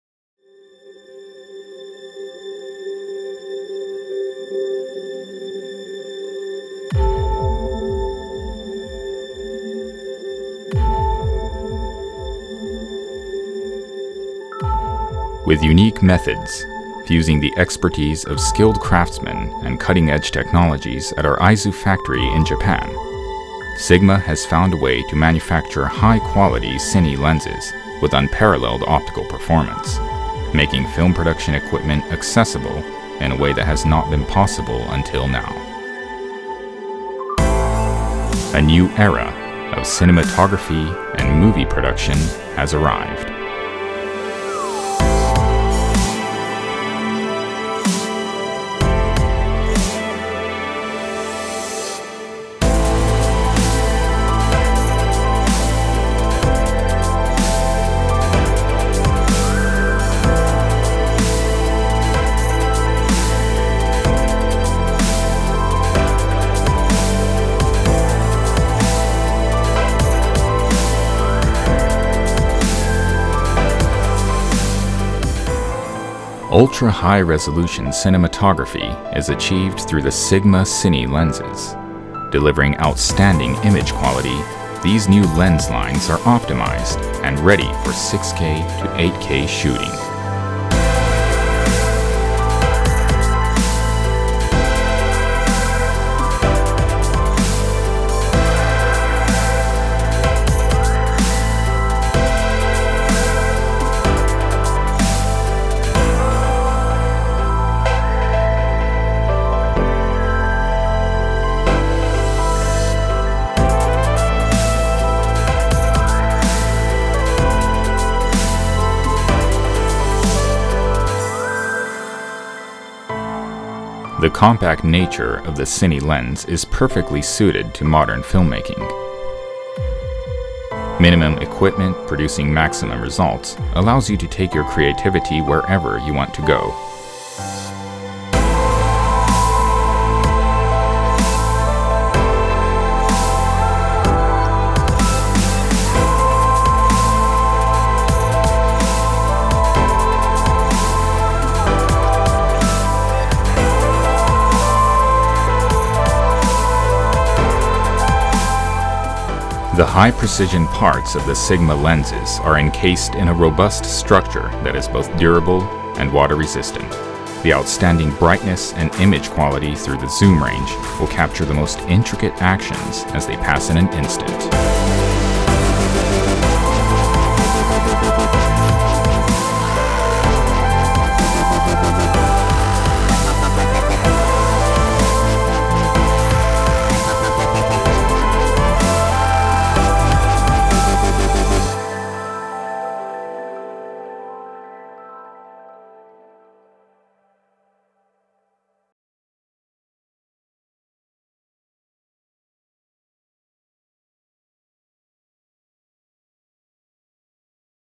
SIGMA CINE LENSーHigh Speed Zoom Line
* Multi language subtitles. French, Spanish and Japanese.

High-performance and compact
Two zoom lens are all you need for professional cinematography.

Ready to take on ultra-high-resolution shoots in 6K-8K, SIGMA’s cine lenses offer both outstanding optical performance and compactness.  Now true cinematography is within reach, even on smaller projects or for special effects.

00:00 Production system
00:52 True cinematography
01:35 Studio shoots for fashion and more
01:51 Underwater photography using housing
02:39 Maneuverability and durability for challenging shoots

Special Thanks
Downhill rider：　Yuki Kushima
Bike Shop Genbu
Fujiten Resort

高性能かつコンパクト。
2本のズームレンズで本格的な撮影が可能に。

SIGMA CINE LENSは、6K～8Kの高解像撮影に対応できる高い光学性能とコンパクトネスを高次元で両立します。小規模撮影や特殊撮影など、あらゆる環境に対応し、本格的な映画撮影の可能性を拡げます。

00:00 生産体制
00:52 本格的な映画撮影
01:35 ファッションシュートなどのスタジオ撮影
01:51 ハウジングを用いた水中撮影
02:39 機動力やタフネスが要求される撮影

撮影協力
ダウンヒルバイクライダー：九島勇気
Bike Shop 玄武
ふじてんリゾート